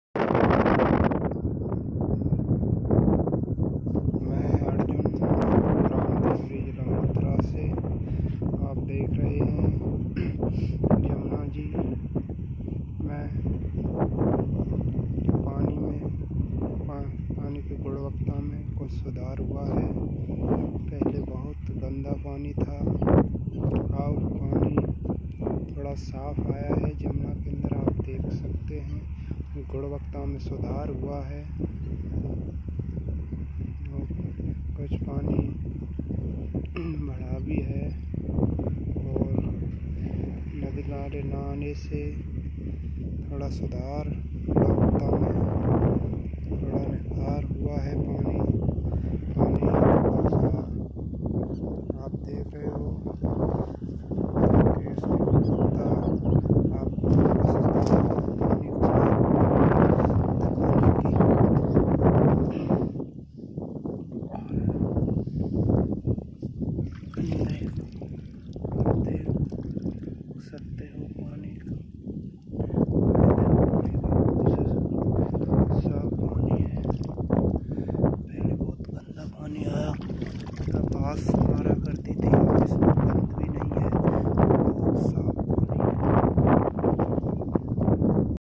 Yamuna Flows Cleaner in Mathura
The non-seasonal rains, snow melt, halt in industrial pollution due to lockdown has increased quality and quantity of water in Yamuna river downstream Okhala Barrage.

The river is seen flowing with improved quality of water. The flow level has also increased.

This video is from Oba village, in Shergah town of Mathura district, Uttar Pradesh.

Video shared by Arjun on April 9, 2020.